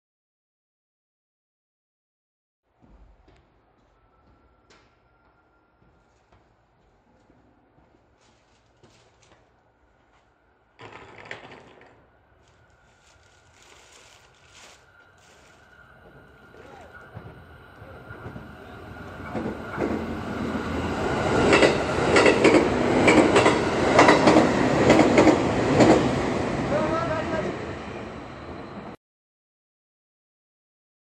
７８３系ＣＭ５ 回送列車 長崎本線神崎駅通過
２０１９・１・４撮影
臨時特急かもめ６４号博多行の送り込みです